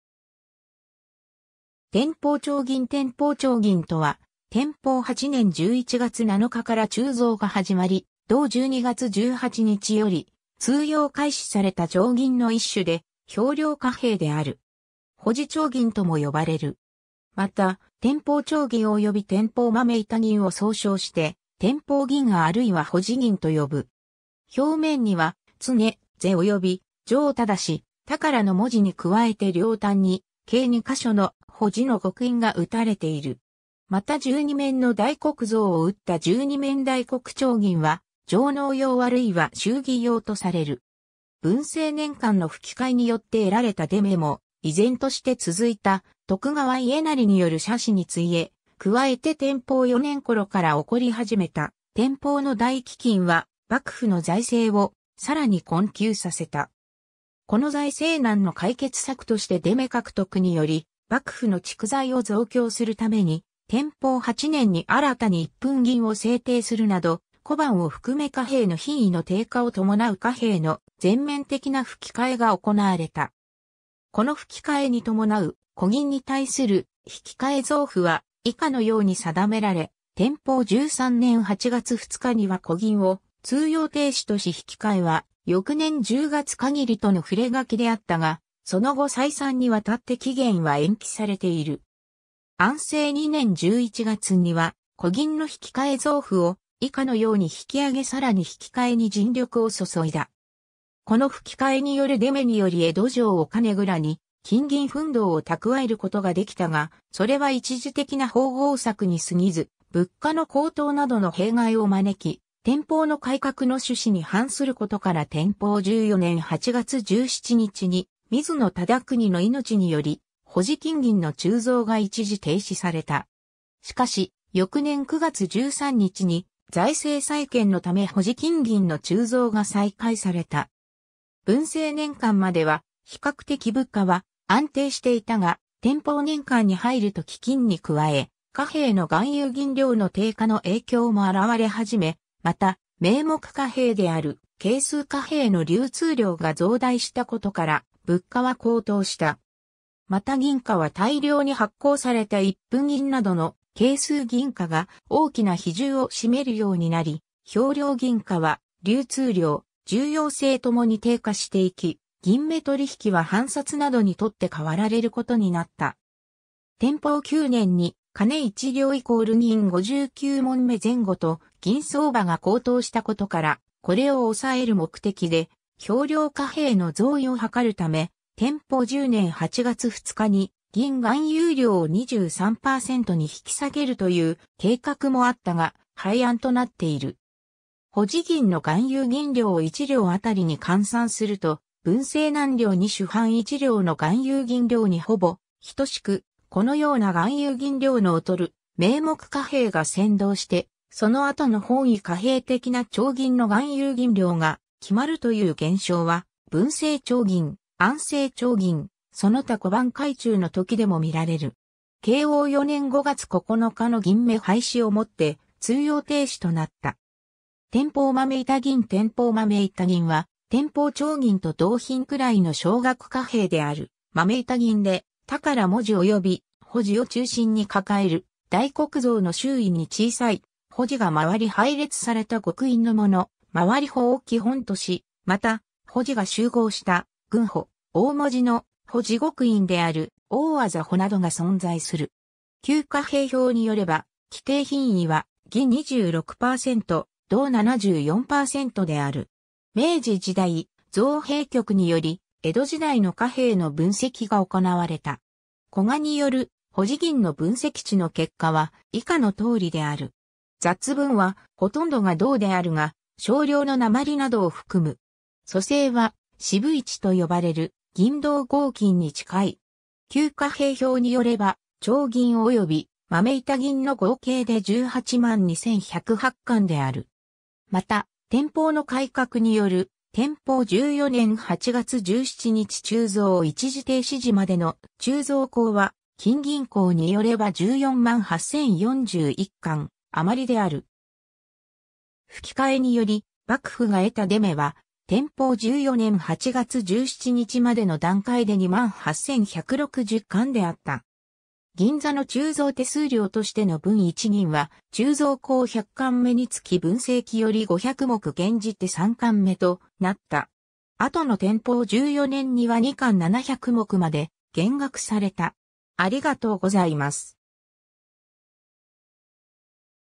天保丁銀
天保丁銀（保字丁銀） 天保丁銀（てんぽうちょうぎん）とは、天保8年11月7日（1837年12月4日）から鋳造が始まり、同12月18日（1838年1月13日）より通用開始された丁銀の一種で秤量貨幣である。
保字丁銀（ほうじちょうぎん/ほじちょうぎん）とも呼ばれる。
また天保丁銀および天保豆板銀を総称して天保銀（てんぽうぎん）あるいは保字銀（ほうじぎん/ほじぎん）と呼ぶ。
 表面には「（大黒像）、常是」および「常是、寳」の文字に加えて両端に計二箇所の「保」字の極印が打たれている。
また12面の大黒像を打った十二面大黒丁銀は上納用あるいは祝儀用とされる。
 文政年間の吹替えによって得られた出目（改鋳利益）も、依然として続いた徳川家斉による奢侈に費え、加えて天保4年（1833年）頃から起こり始めた天保の大飢饉は幕府の財政をさらに困窮させた。
この財政難の解決策として出目（改鋳利益）獲得により幕府の蓄財を増強するために、天保8年（1837年）に新たに一分銀を制定するなど、小判を含め貨幣の品位の低下を伴う貨幣の全面的な吹替えが行われた。
 この吹替えに伴う古銀に対する引替増歩は以下のように定められ、天保13年8月2日（1842年9月6日）には古銀を通用停止とし引替は翌年10月限りとの触書であったが、その後再三に亘って期限は延期されている。
 安政2年11月（1855年12月頃）には、古銀の引替増歩を以下のように引き上げさらに引替に尽力を注いだ。
 この吹替えによる出目により江戸城御金蔵に金銀分銅を蓄えることができたが、それは一時的な縫合策に過ぎず、物価の高騰などの弊害を招き、天保の改革の趣旨に反することから天保14年8月17日（1843年9月10日）に水野忠邦の命により保字金銀の鋳造が一時停止された。
しかし翌年9月13日（1844年10月24日）に財政再建のため保字金銀の鋳造が再開された。
 文政年間までは比較的物価は安定していたが、天保年間に入ると飢饉に加え、貨幣の含有銀量の低下の影響も現れ始め、また名目貨幣である計数貨幣の流通量が増大したことから物価は高騰した。
また銀貨は大量に発行された一分銀などの計数銀貨が大きな比重を占めるようになり、秤量銀貨は流通量、重要性ともに低下していき、銀目取引は藩札などに取って代わられることになった。
天保9年（1838年）に金一両＝銀五十九匁前後と銀相場が高騰したことから、これを抑える目的で秤量貨幣の増鋳を図るため、天保10年8月2日（1839年9月9日）に銀含有量を23%に引き下げるという計画もあったが、廃案となっている。
 保字銀の含有銀量を一両当りに換算すると、文政南鐐二朱判一両の含有銀量にほぼ等しく、このような含有銀量の劣る名目貨幣が先導して、その後の本位貨幣的な丁銀の含有銀量が決まるという現象は文政丁銀、安政丁銀、その他小判改鋳のときでも見られる。
慶應4年5月9日（1868年6月28日）の銀目廃止をもって通用停止となった。
 天保豆板銀（保字小玉銀） 天保豆板銀（てんぽうまめいたぎん）は天保丁銀と同品位の少額貨幣である豆板銀で、「寳」文字および「保」字を中心に抱える大黒像の周囲に小さい「保」字が廻り配列された極印のもの「廻り保」を基本とし、また「保」字が集合した「群保」、大文字の「保」字極印である「大字保」などが存在する。
 『旧貨幣表』に依れば、規定品位は銀26%（七割一分四厘引ケ）、銅74%である。
 明治時代、造幣局により江戸時代の貨幣の分析が行われた。
古賀による保字銀の分析値の結果は以下の通りである。
 雑分はほとんどが銅であるが、少量の鉛などを含む。
組成は四分一（しぶいち）と呼ばれる銀銅合金に近い。
 『旧貨幣表』によれば、丁銀および豆板銀の合計で182,108貫（約680トン）である。
 また天保の改革による天保14年8月17日（1843年9月10日）鋳造一時停止時までの鋳造高は、『金銀考』によれば148,041貫余である。
 吹替えにより幕府が得た出目（改鋳利益）は天保14年8月17日（1843年9月10日）までの段階で28,160貫であった。
 銀座の鋳造手数料としての分一銀は、鋳造高100貫目につき文政期より500目減じて3貫目となった。
後の天保14年（1843年）には2貫700目まで減額された。